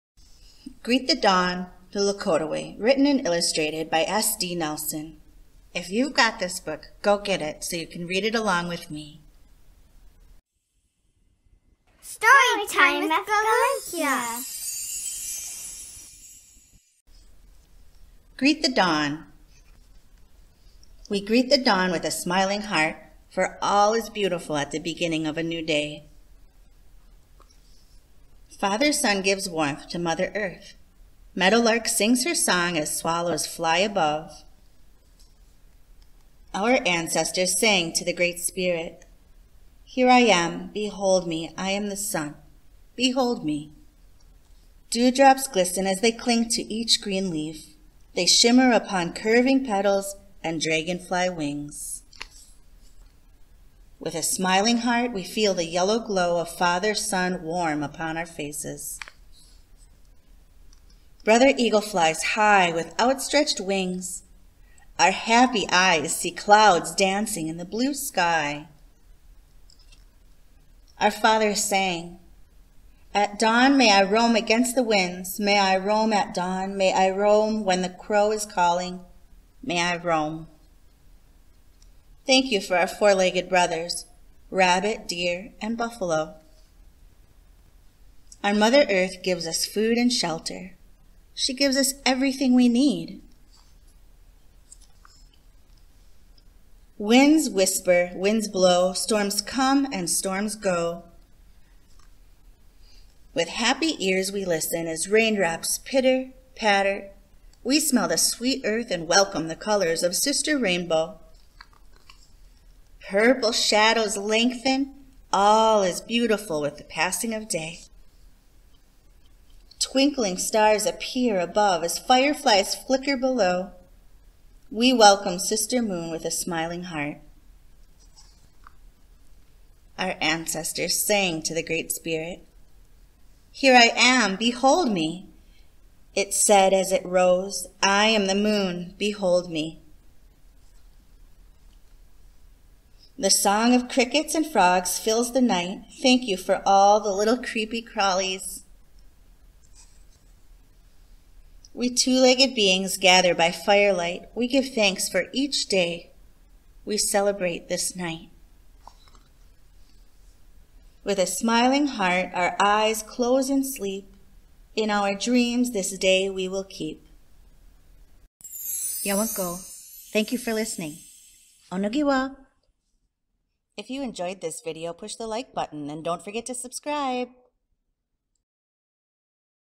Greet the Dawn Read by Kaluhya
A Lakota tradition of gratitude.